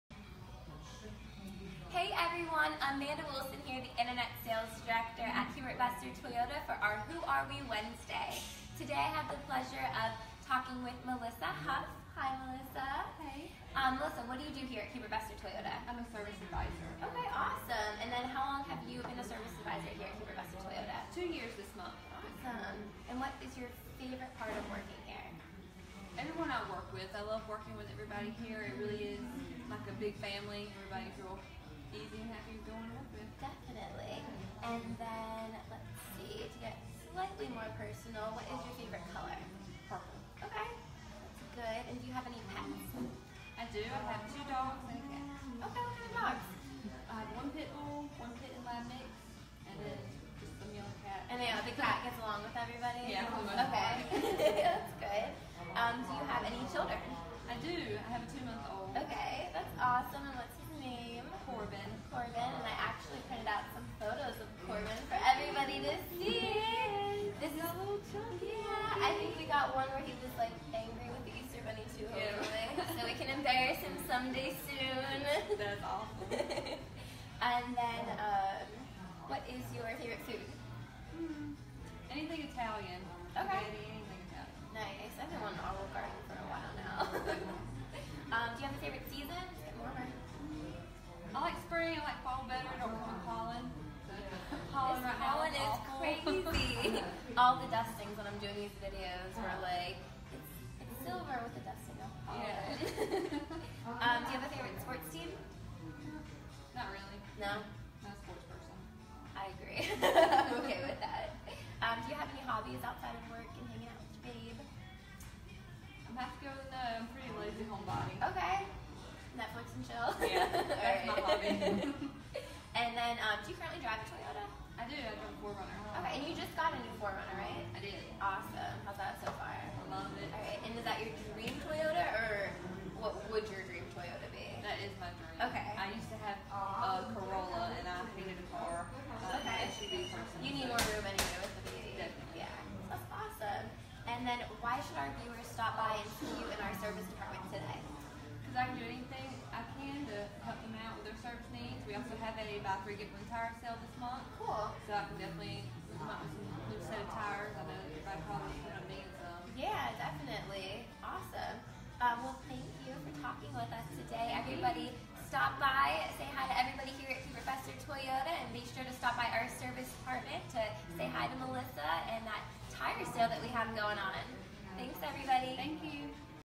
Who Are We Wednesday Featuring Melissa Hough
We LOVE our team here at Hubert Vester Toyota!

Each Wednesday we wanted to take the time to introduce you to a member of our team!